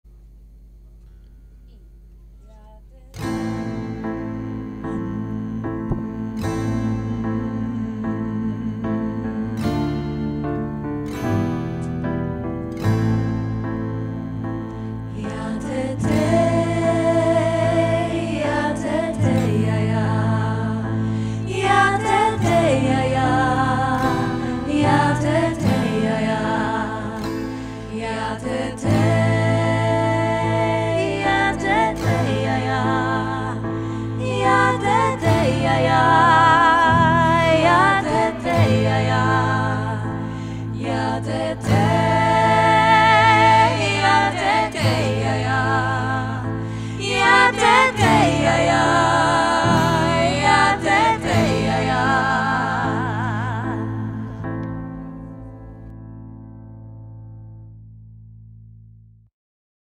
Dla Pani Eli, bardzo niezwykła niespodzianka :)
Wakacyjna pamiątka, dla naszej Pani Eli zorganizowana przez warsztatowiczów półkolonijnych ;)
Cover zespołu Łowcy Dźwięków